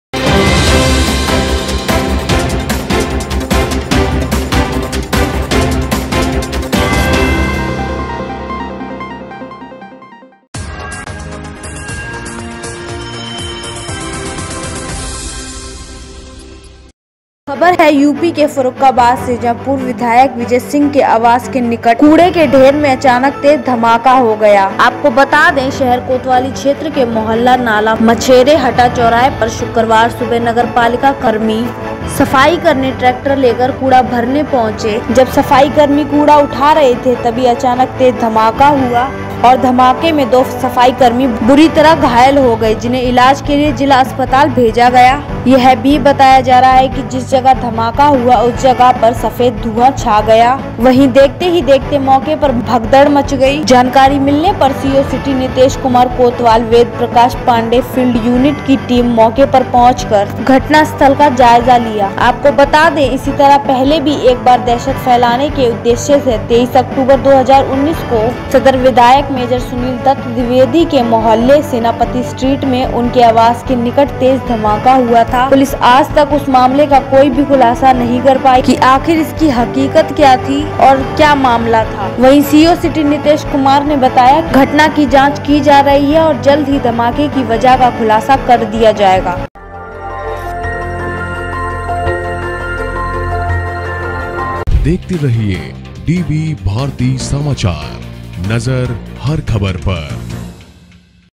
फ़र्रूख़ाबाद उ०प्र० खबर
कूड़े के ढेर में बम मिलने से दो सफाई कर्मी घायल पूर्व विधायक के आवास के निकट कूड़े के ढेर मैं अचानक तेज धमाका कूड़ा भर रहे दो नगर पालिका सफाई कर्मी जख्मी पुलिस ने मौके पर जाकर जांच पड़ताल शुरू की शहर कोतवाली क्षेत्र के मोहल्ला नाला मछरेहटा चौराहे का मामला.